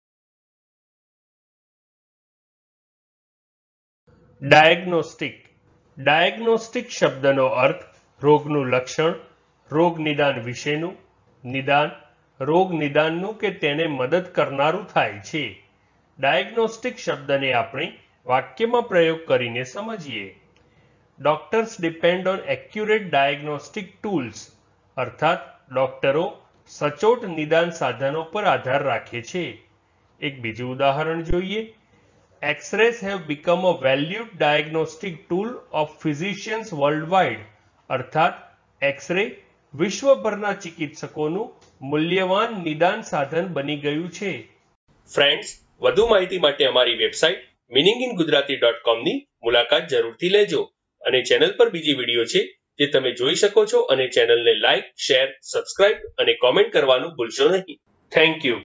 Diagnostic Meaning in Gujarati | Diagnostic નો અર્થ શું છે | Diagnostic in Gujarati Dictionary
આ વિડિયો માં તમે Diagnostic નો Gujarati માં અર્થ સમજશો અને એની સાથે Diagnostic નું pronunciation પણ શિખશો. એટ્લે કે તમે Diagnostic ના meaning ની સાથે સાથે એ પણ શિખશો કે Diagnostic ને કેવી રીતે બોલાય, Diagnostic ને બોલવાની સાચી રીત કઈ છે.

તો બસ એક મિનટ માં શીખો Diagnostic શબ્દ ને.

Diagnostic ને examples થી સમજીએ